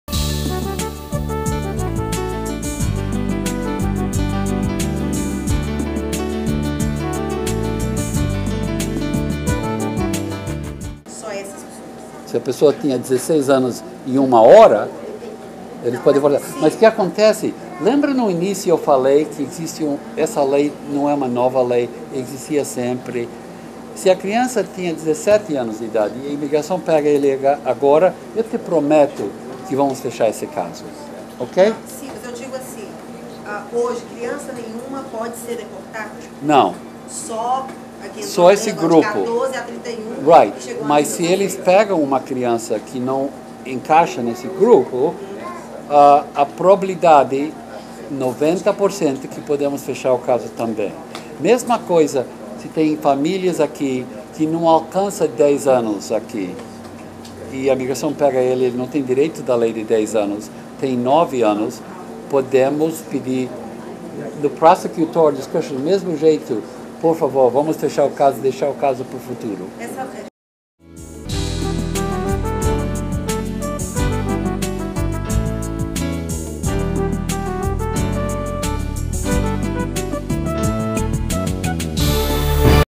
Dream Act - Perguntas e respostas 17
Pergunta:
Crianças que não se qualificam para o
Dream Act, podem ser beneficiadas
também ?